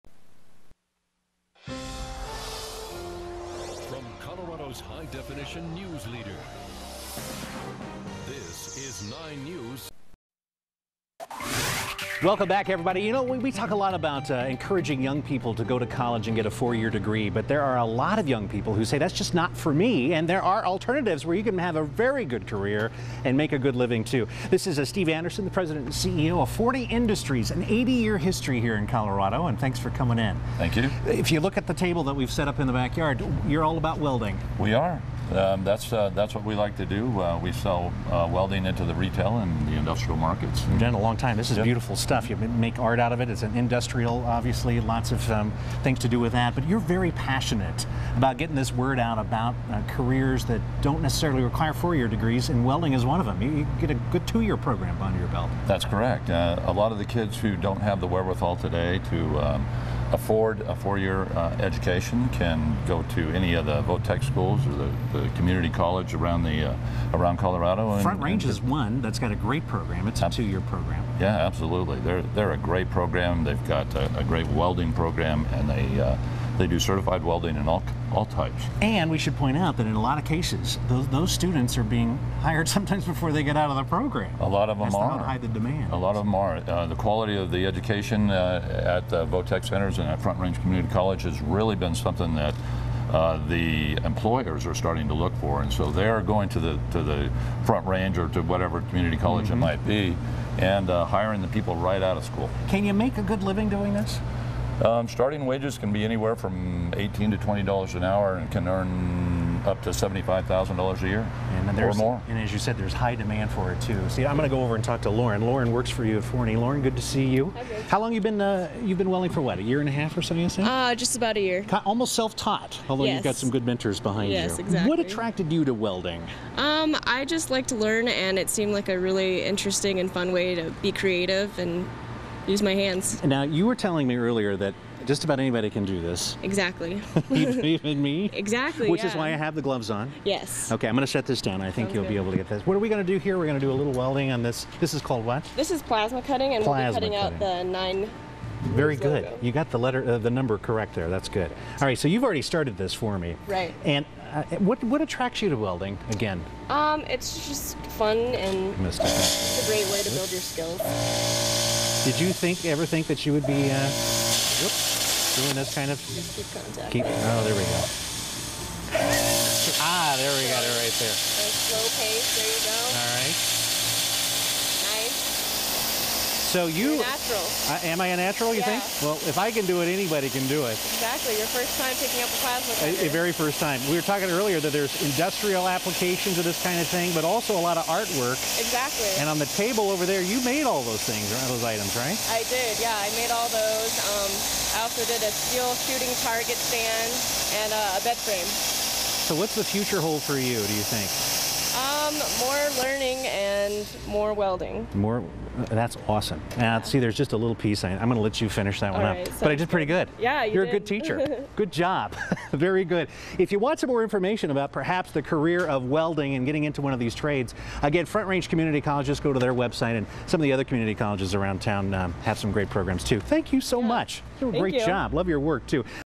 Welding: A Hands-On Career in High Demand
KUSA - College isn't always the best option for students looking to build a career after high school.
Have questions?